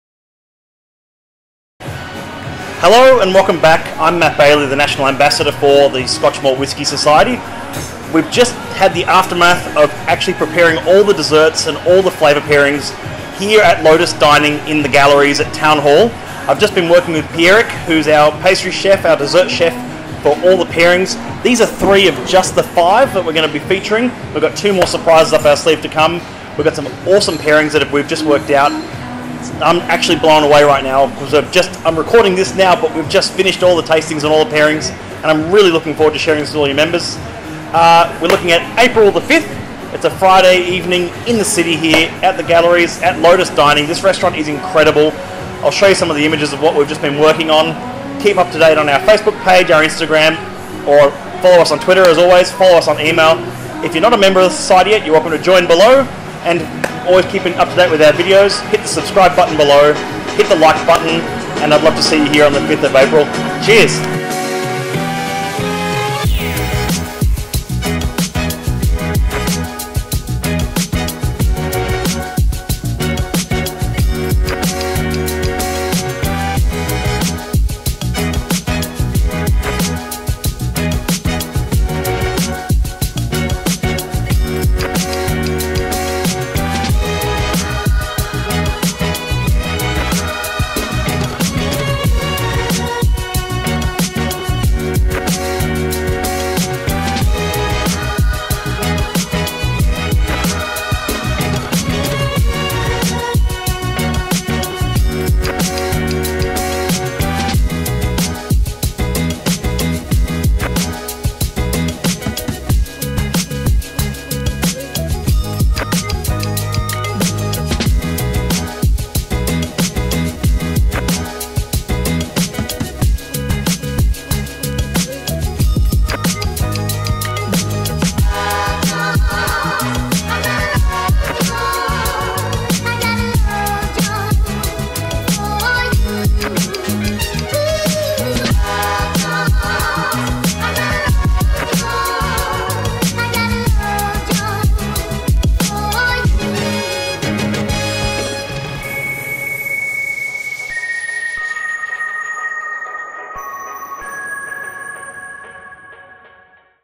Dessert & Whisky Pairing: Lotus Dining Taste Test Session!
Matt Bailey caught up with Pierrick Nicol to taste test some of the single cask whisky desserts we're working on for the 5th of April 2019 at Lotus Dining, The Galeries, Sydney. Single cask whiskies expertly paired with hand-crafted desserts, for one night only in Sydney.